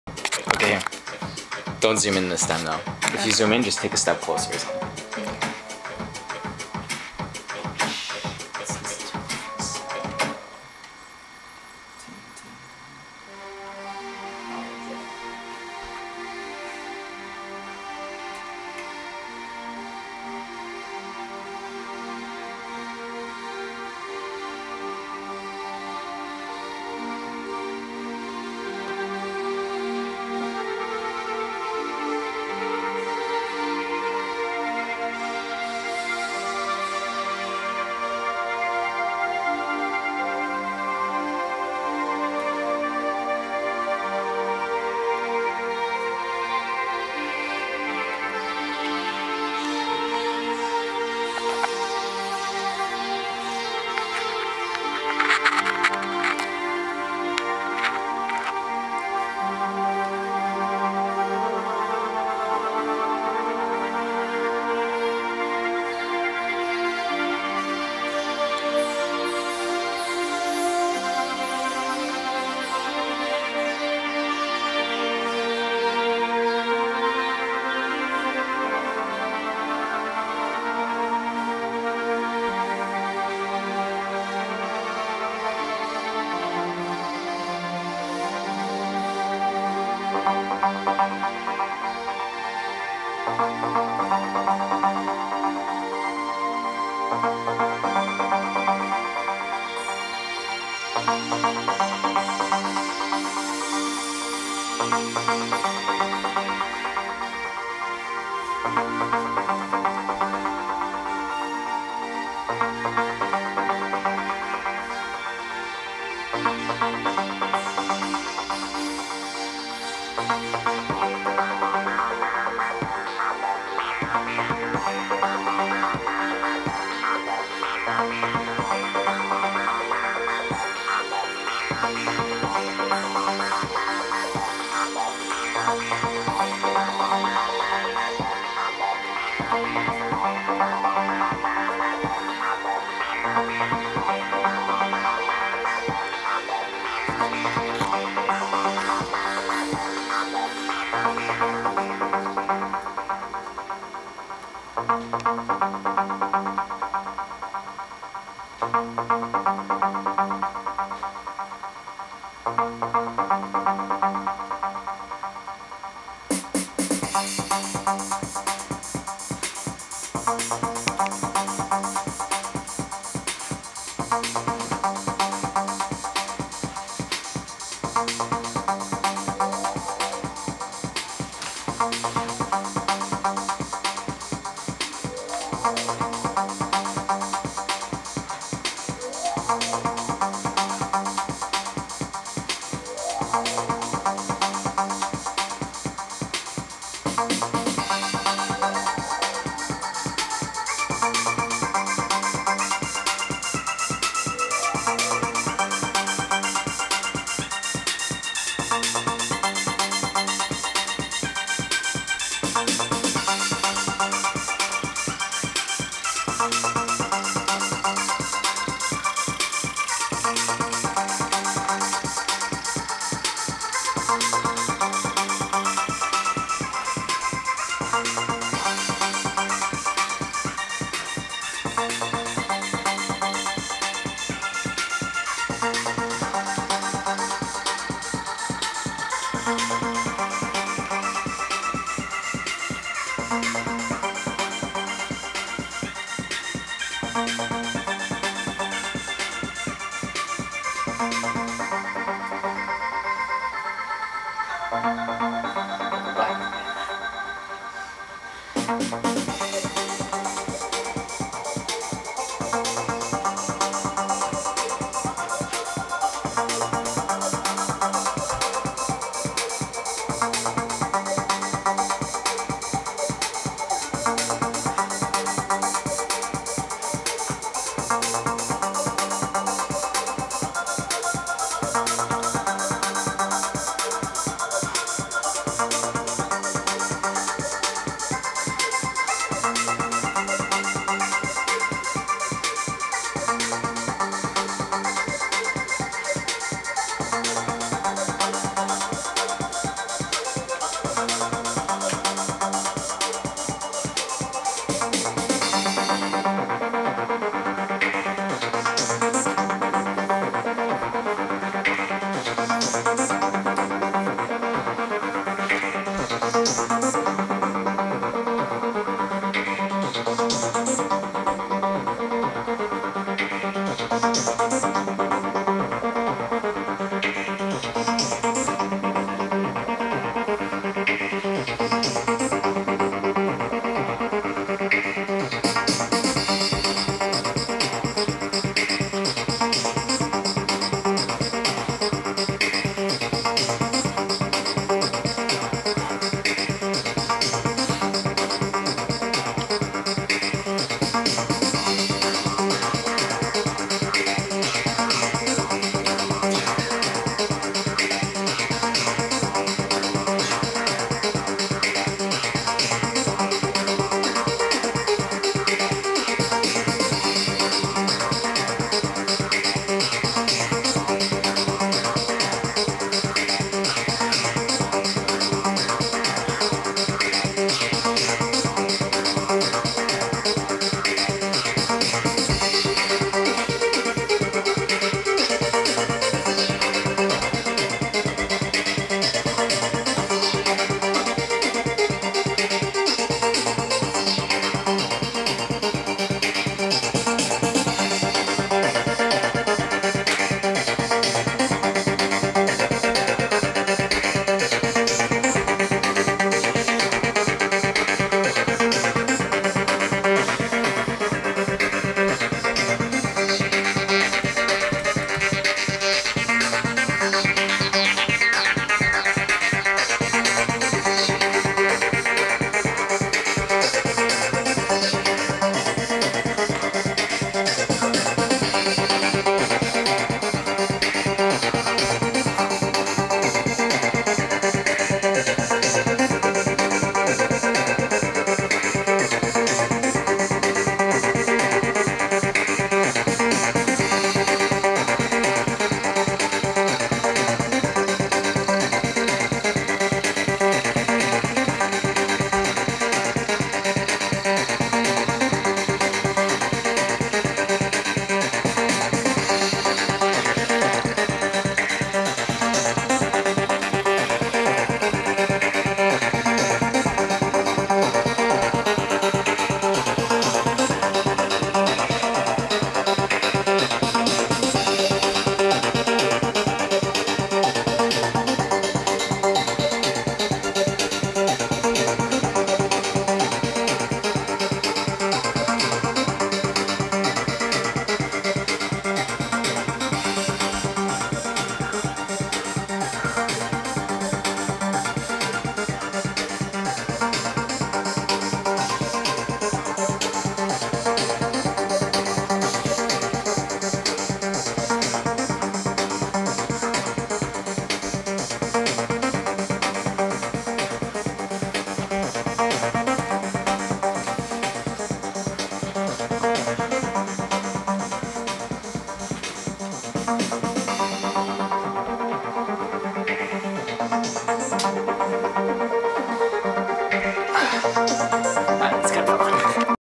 Liquid Dance
Constructive criticism welcome!
This was my first time dancing while high in over a year. Sorry for crazy camra girl